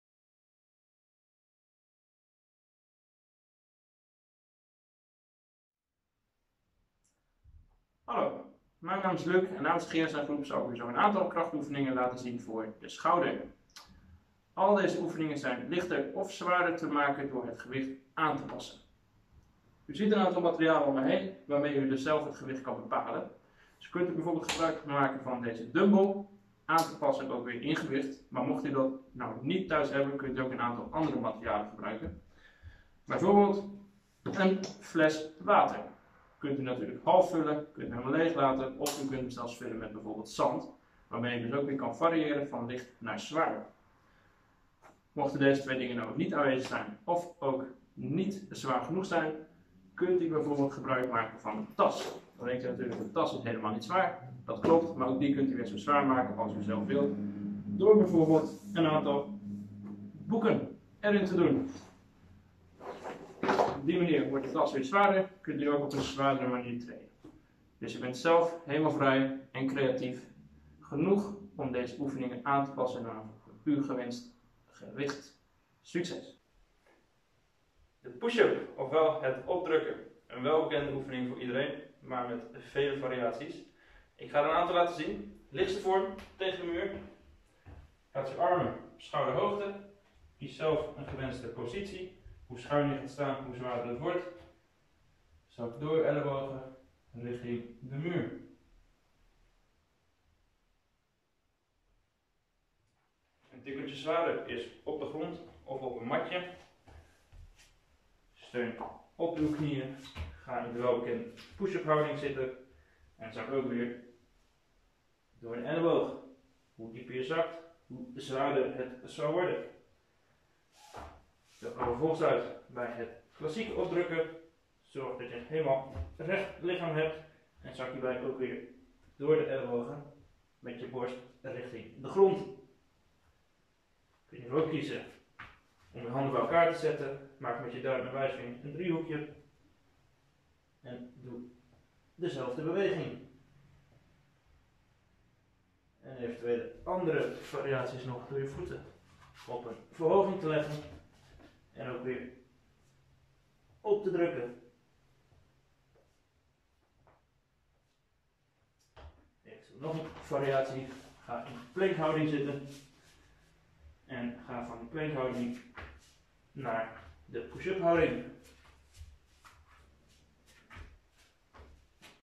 Schouder oefeningen - Push Up
Luuk Biemans neemt je mee in diverse krachtsoefeningen voor de schouder die thuis prima uitgevoerd kunnen worden. Hiervoor kan je diverse hulpmiddelen gebruiken om mee te trainen. Door aanpassen van gewicht of voorwerp kan de de zwaarte van de oefeningen naar eigen inzichten aanpassen en op eigen niveau uitvoeren.
De volgende oefeningen worden besproken:
- bicepscurl
- frontraise
- side raise
- triceps extensie
- push up
- overhead press

Probeer alle oefeningen uit en laat weten wat je van de oefeningen vond